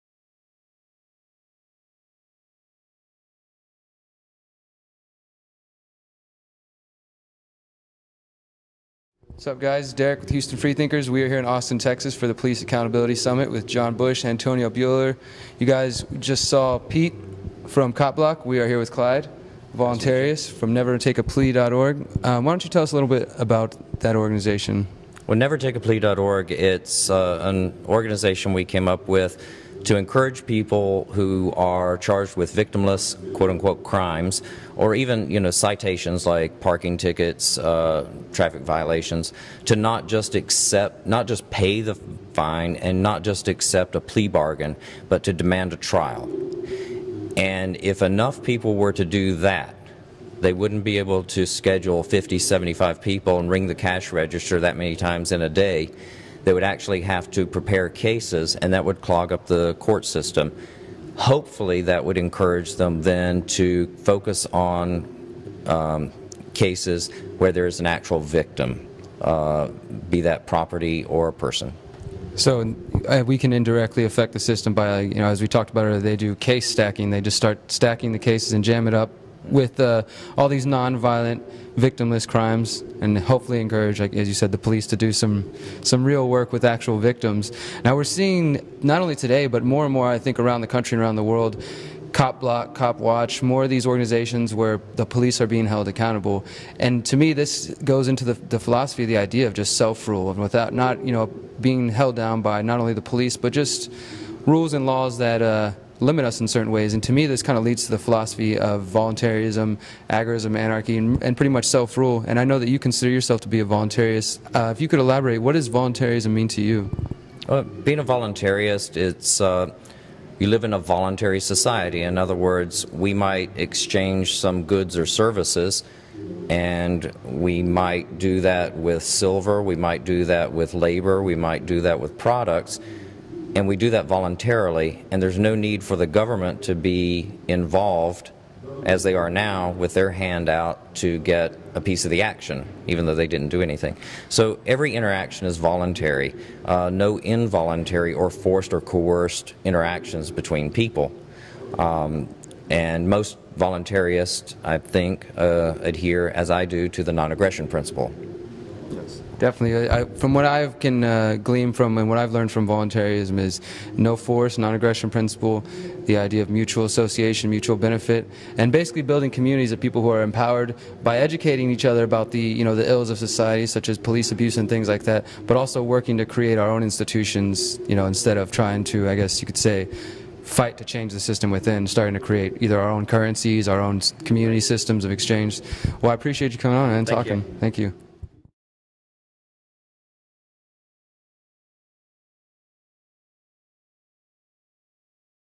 HFT Interviews Clyde Voluntaryist of NeverTakeAPlea.org @The Police Accountability Summit
HFT Interviews Clyde Voluntaryist of NeverTakeAPlea.org @The Police Accountability Summit in Austin, TX